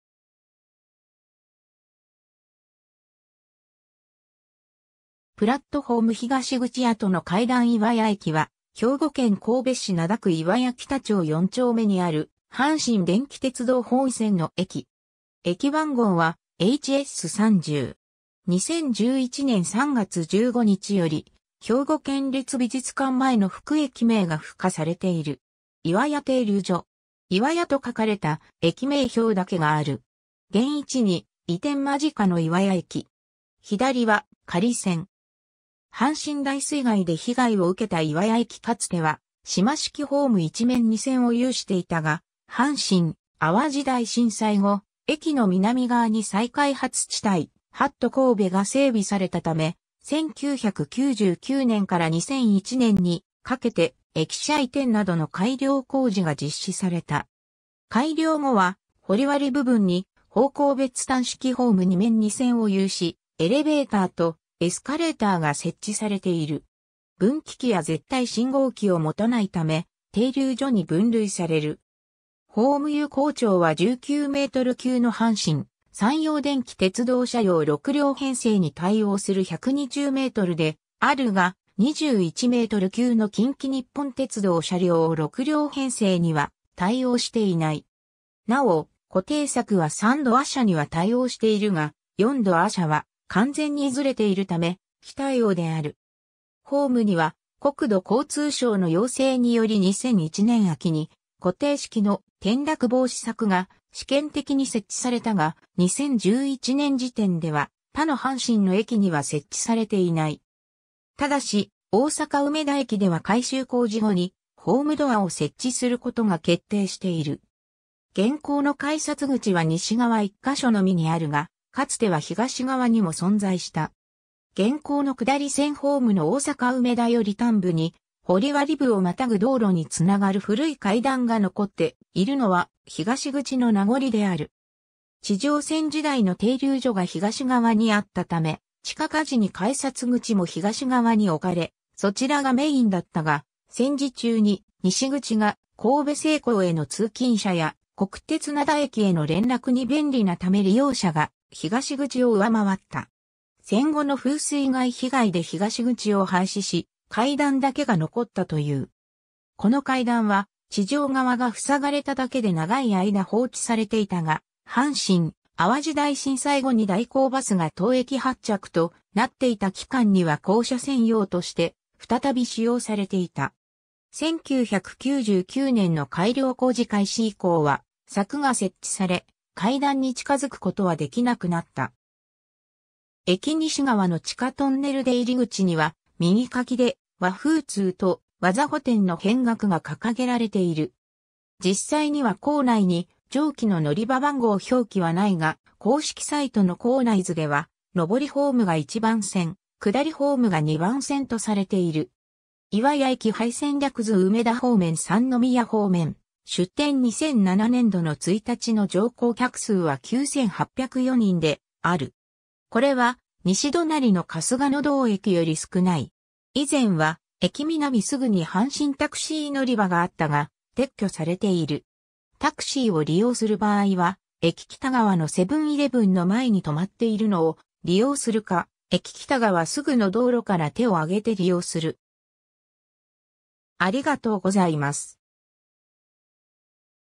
岩屋駅 (兵庫県)
プラットホーム（2010年2月21日） 東口跡の階段（2012年6月10日） 岩屋駅（いわやえき）は、兵庫県神戸市灘区岩屋北町四丁目にある、阪神電気鉄道本線の駅。
駅番号はHS 30。
2011年3月15日より「兵庫県立美術館前」の副駅名が付加されている。
 岩屋停留所。
「いはや」と書かれた駅名標だけがある。
（撮影年不明） 現位置に移転間近の岩屋駅。
左は仮線。
（1933年） 阪神大水害で被害を受けた岩屋駅(1938年) かつては島式ホーム1面2線を有していたが、阪神・淡路大震災後、駅の南側に再開発地帯「HAT神戸」が整備されたため、1999年から2001年にかけて駅舎移転などの改良工事が実施された。
改良後は掘割部分に方向別単式ホーム2面2線を有し、エレベーターとエスカレーターが設置されている。
ホーム有効長は19m級の阪神・山陽電気鉄道車両6両編成に対応する120mであるが、21m級の近畿日本鉄道車両6両編成には対応していない。
なお、固定柵は3ドア車（阪神列車と、山陽列車）には対応しているが、4ドア車（近鉄列車）は完全にずれているため、非対応である。
 ホームには、国土交通省の要請により2001年秋に固定式の「転落防止柵」が試験的に設置されたが、2011年時点では他の阪神の駅には設置されていない。
但し、大阪梅田駅では改修工事後に、ホームドアを設置することが決定している。
 現行の改札口は西側1か所のみにあるが、かつては東側にも存在した。
現行の下り線ホーム（かつての島式ホーム）の大阪梅田寄り端部に掘割部を跨ぐ道路に繋がる古い階段が残っているのは、東口の名残である。
 地上線時代の停留所が東側にあったため地下化時に改札口も東側に置かれ、そちらがメインだったが、戦時中に西口が神戸製鋼への通勤者や国鉄灘駅への連絡に便利なため利用者が東口を上回った。
戦後の風水害被害で東口を廃止し、階段だけが残ったという。
この階段は地上側が塞がれただけで長い間放置されていたが、阪神・淡路大震災後に代行バスが当駅発着となっていた期間には降車専用として再び使用されていた。
1999年の改良工事開始以降は柵が設置され、階段に近付く事は出来なくなった。
 駅西側の地下トンネル出入口には右書きで「和風通」（三宮方面）と「技補天」（梅田・大阪難波方面）の扁額が掲げられている。
 岩屋駅配線略図 ←梅田方面　三宮方面→ 出典 2007年度の1日の乗降客数は9,804人である。
これは西隣の春日野道駅より少ない。
 以前は駅南すぐに阪神タクシー乗り場があったが、撤去されている。
タクシーを利用する場合は駅北側のセブン-イレブンの前に止まっているのを利用するか（タクシー乗り場の看板がないため本来の乗り場ではない）、駅北側すぐの道路から（頻繁に通過している。本来はこちらの方の利用を推奨）手を上げて利用する。